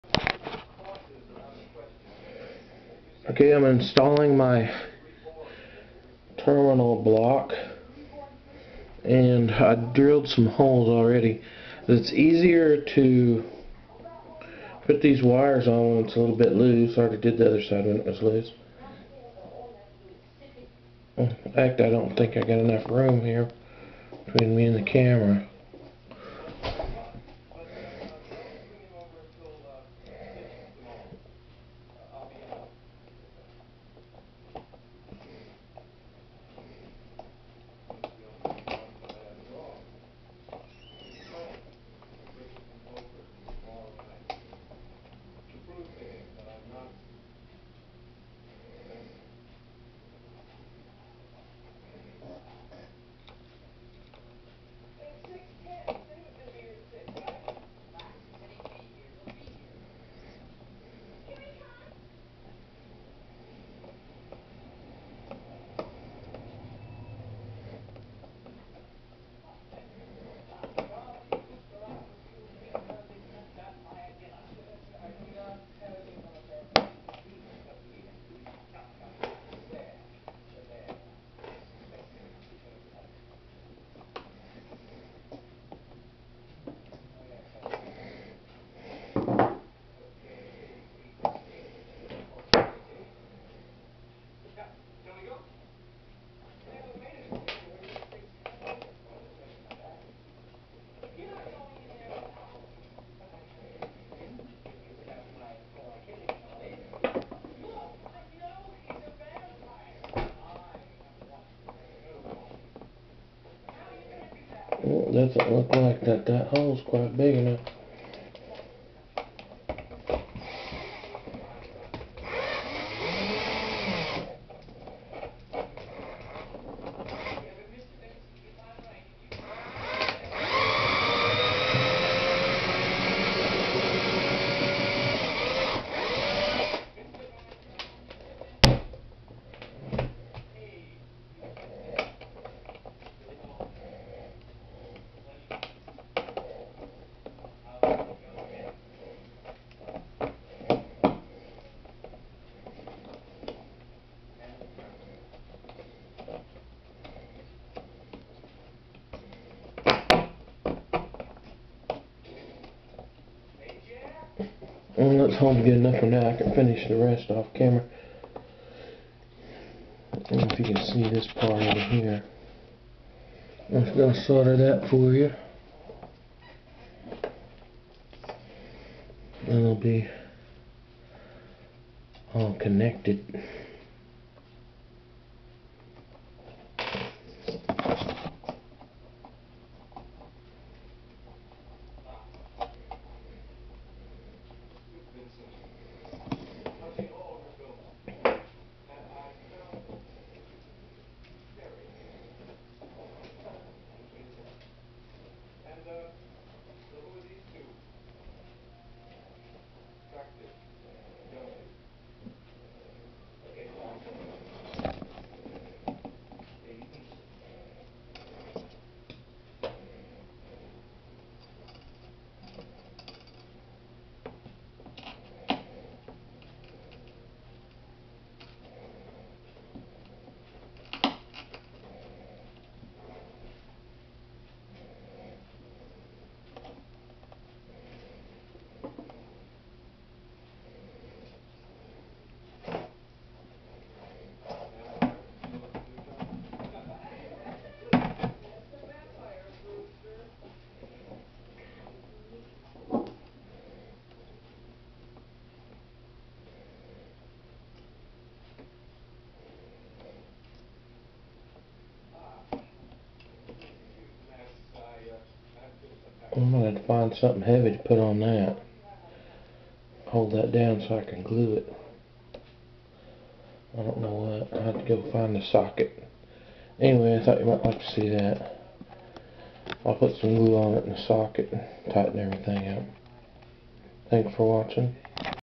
How to Make a Solar Panel That Will Last For Thirty Years Part 10
I show how to install a terminal Block for a aluminum framed solar panel.